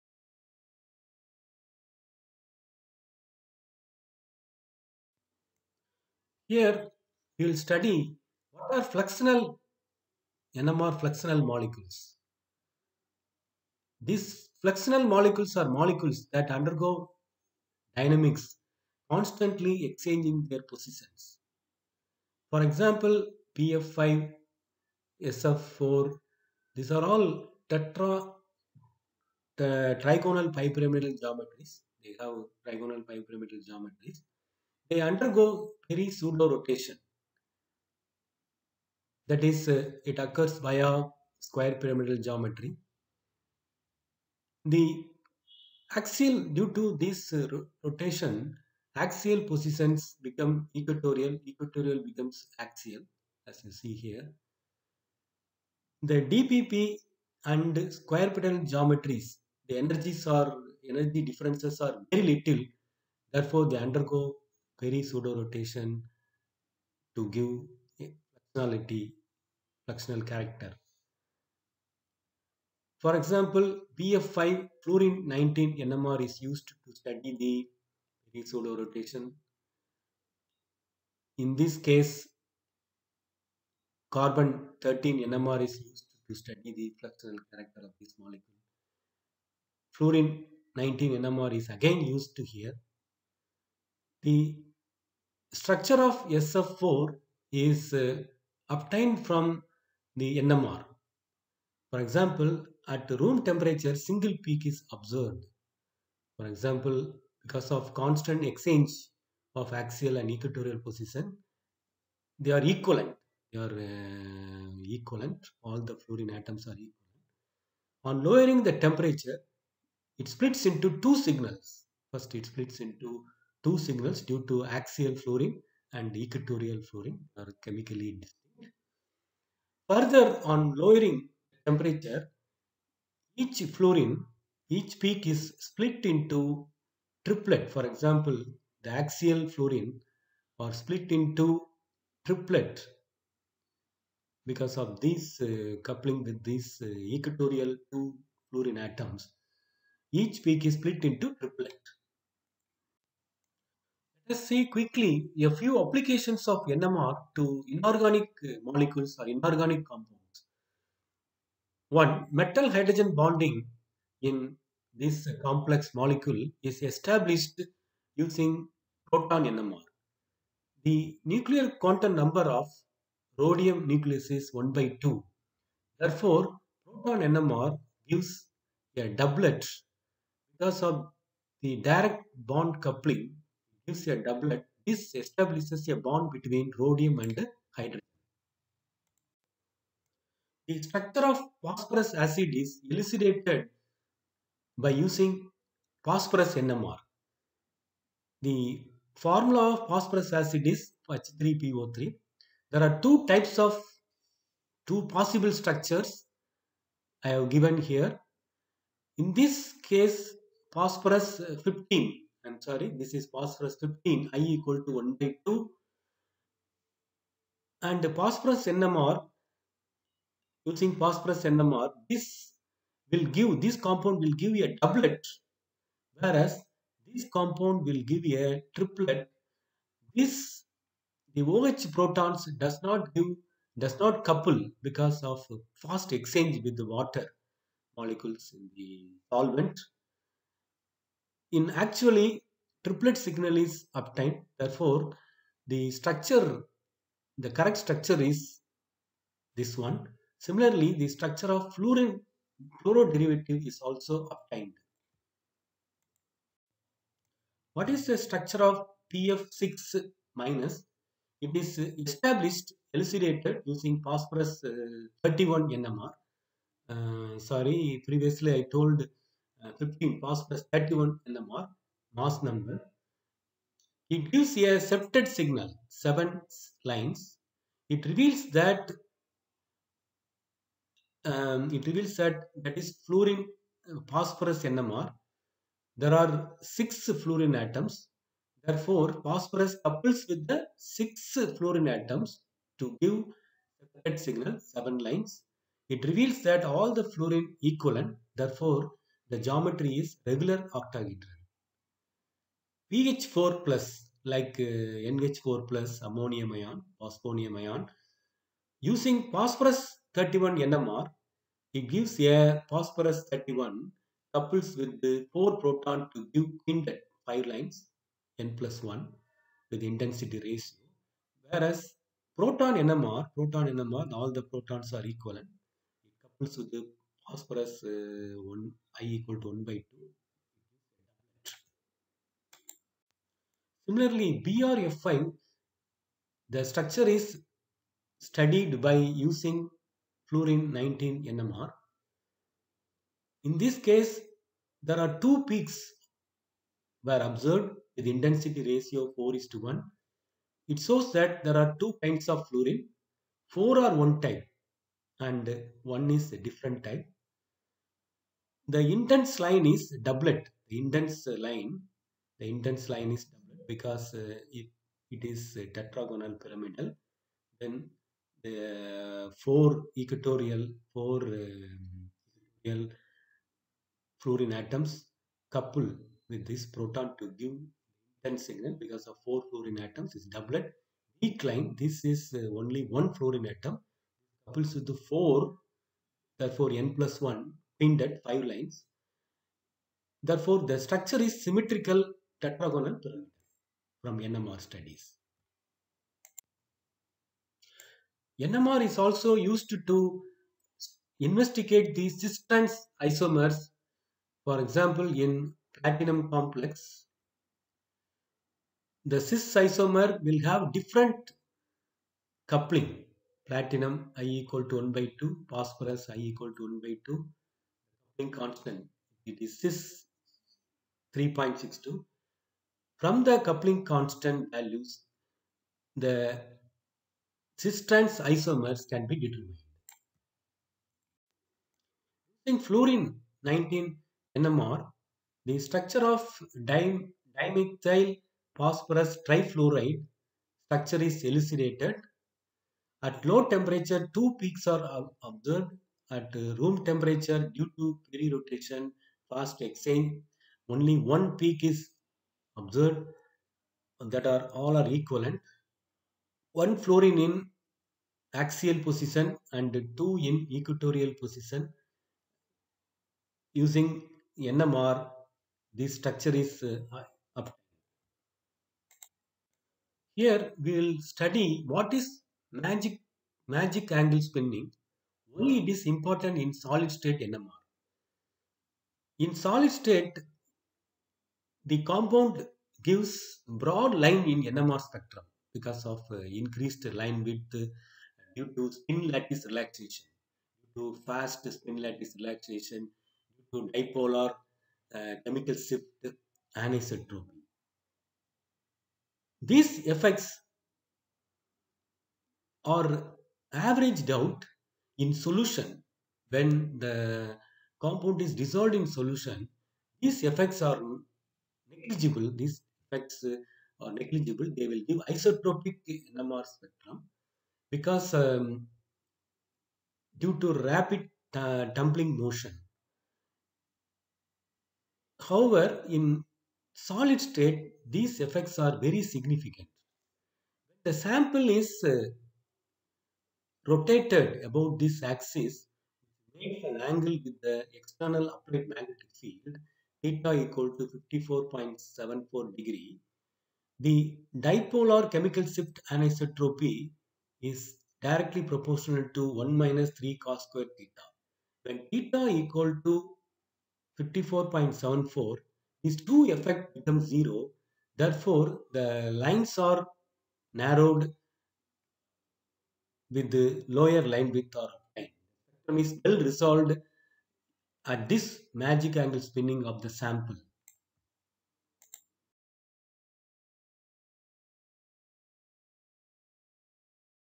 (ENGLISH) NMR APPLICATIONS INORGANIC COMPOUNDS BERRY PSEUDO ROTATION DYNAMICS & MAGIC ANGLE SPINNING
(ENGLISH) NMR APPLICATIONS TO INORGANIC COMPOUNDS, BERRY PSEUDO ROTATION DYNAMICS & MAGIC ANGLE SPINNING SOLID STATE NMR, 19F  31P  14N C13 PROTON NMR STRUCTURE ELUCIDATION